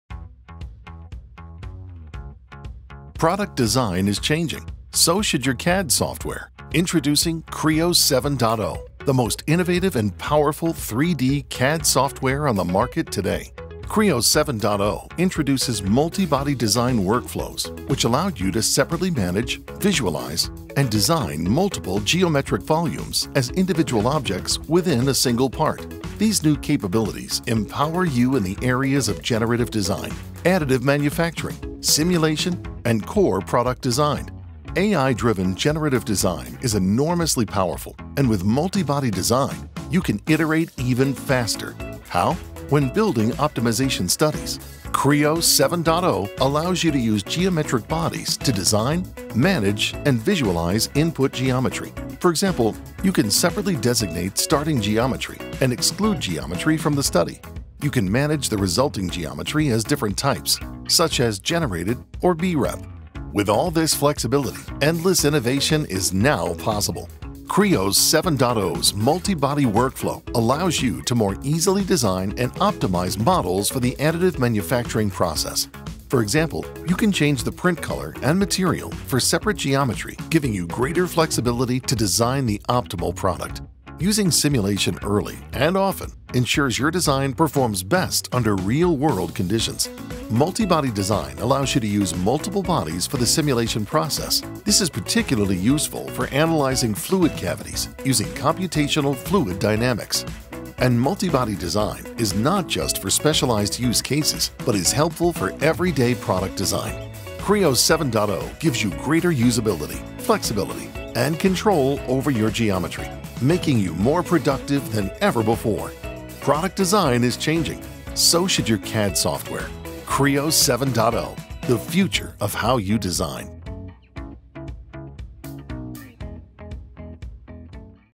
Creo 7.0 - Multibody Design
Creo 7.0 introduces multibody design workflows which allow you to separately manage, visualize, and design multiple geometric volumes as individual objects within a single part. These new capabilities empower you in the areas of generative design, additive manufacturing, simulation, and core product design.